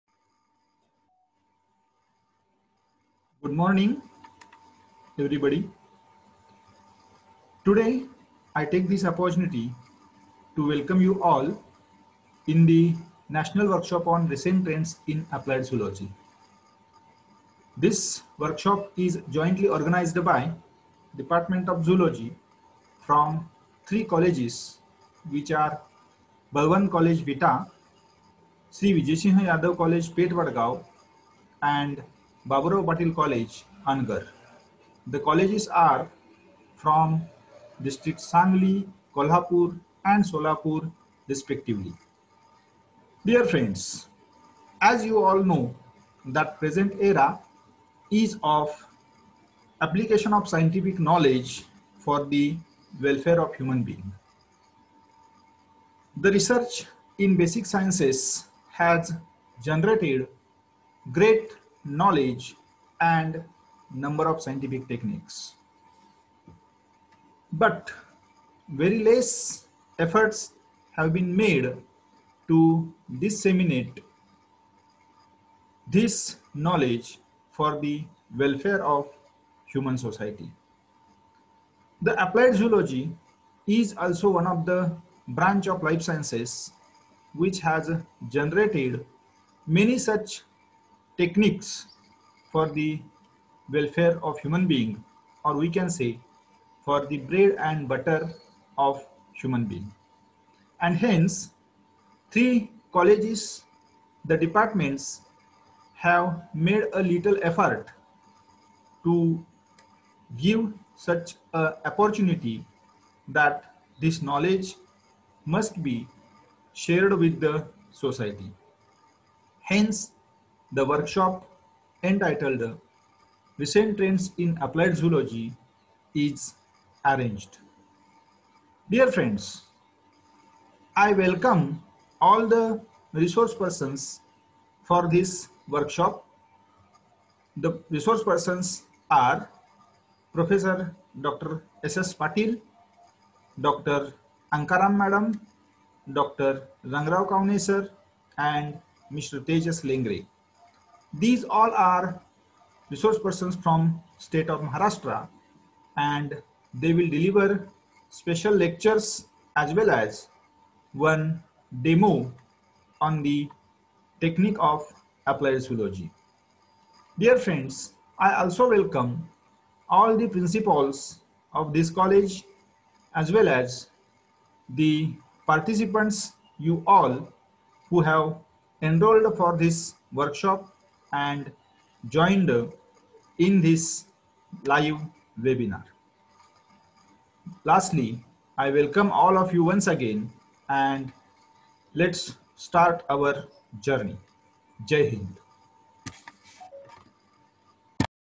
Welcome Preface by Dr. Rajaram Atigre
Welcome Preface by Dr. Rajaram Atigre, Asst. Professor and Head Department of Zoology, Shri. Vijaysinha Yadav College, Peth Vadgaon,
                        One Day National Workshop on Recent Trends In Applied Zoology, Jointly Organized by Department of Zoology of Balwant College, Vita, Dist. Sangli. Shri. Vijaysinha Yadav College, Peth Vadgaon, Dist. Kolhapur. And Baburao Patil College, Anagar Dist. Solapur.